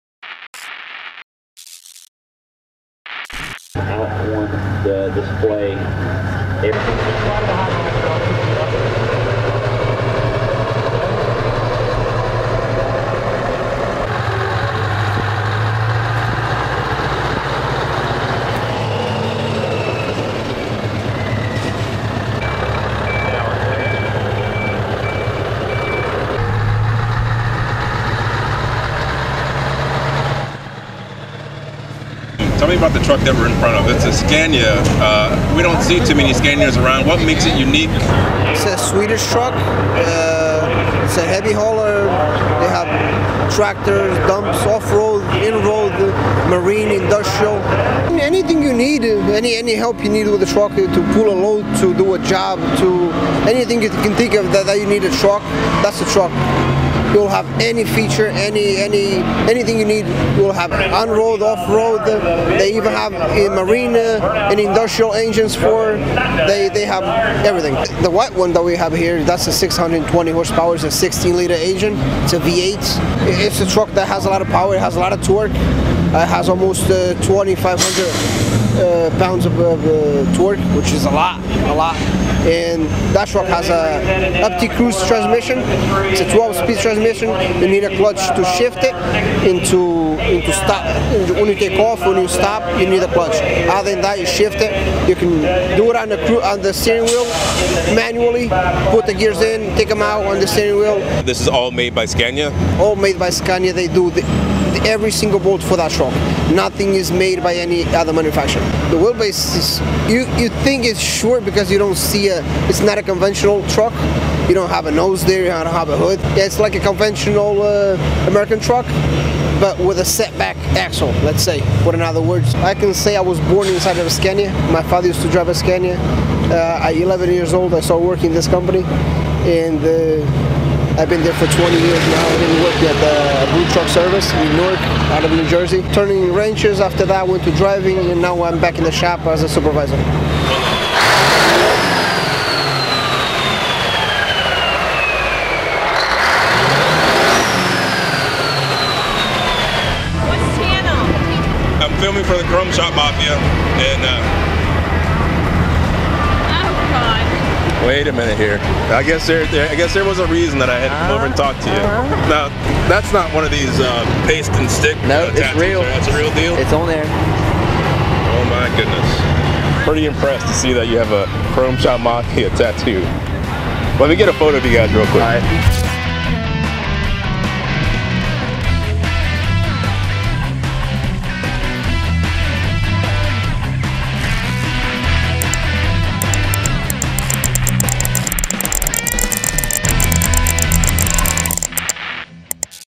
Owner Operator Interview - Abreu Truck Services
Manny Brito of Abreu Truck Services out of Newark, New Jersey talks about the Scania Trucks they brought to the 2013 Rollin' Coal Truck Show in Virginia. Also a Chrome Shop Mafia faithful Josh Valor was spotted sporting some CSM ink.

Produced By: Christopher E. Fiffie